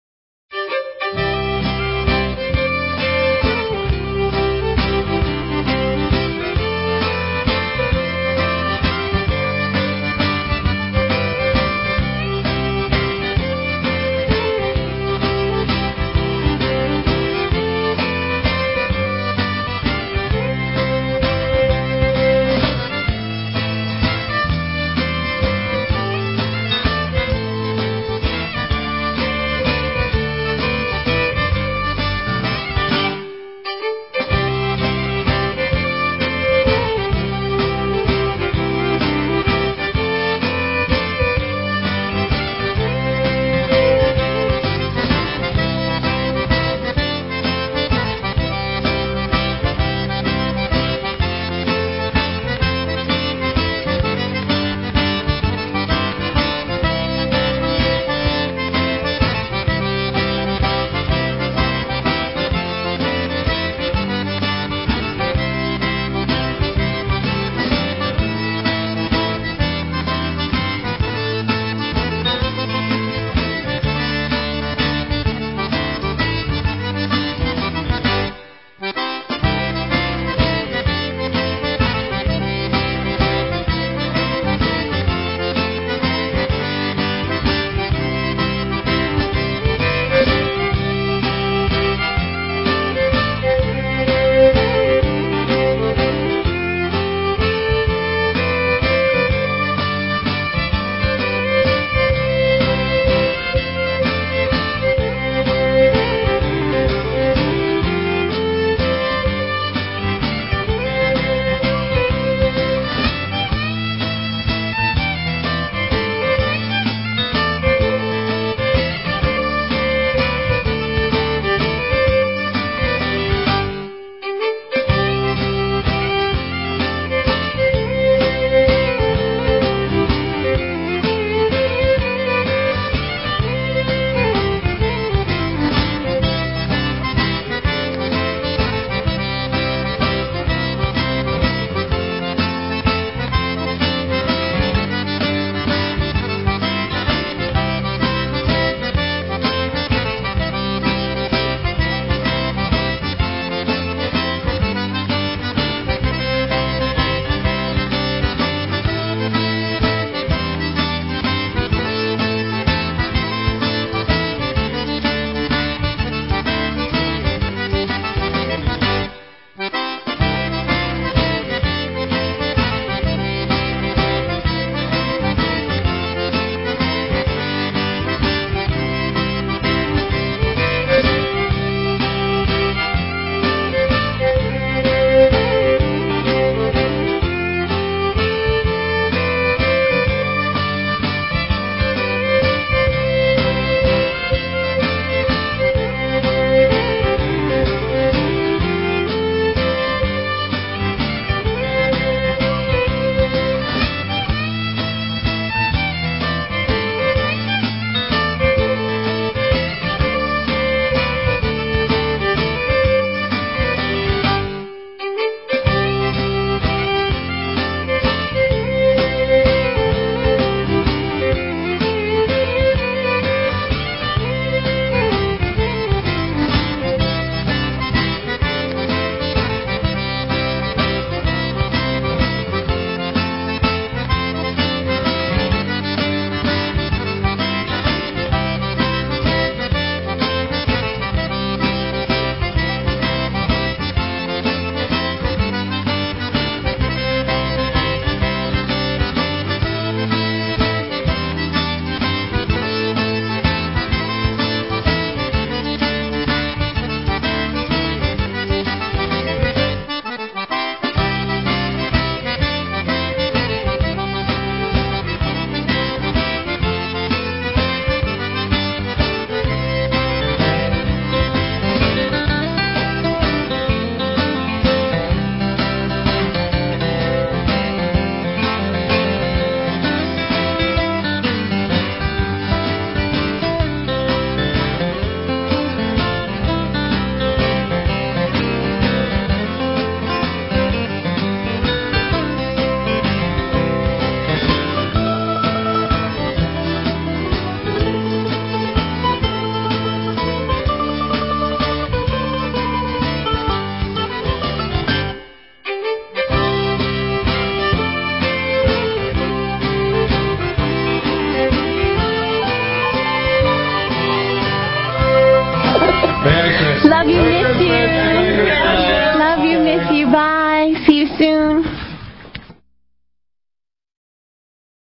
Gruben Christmas 2006
Slide show of Gruben family Christmas get together, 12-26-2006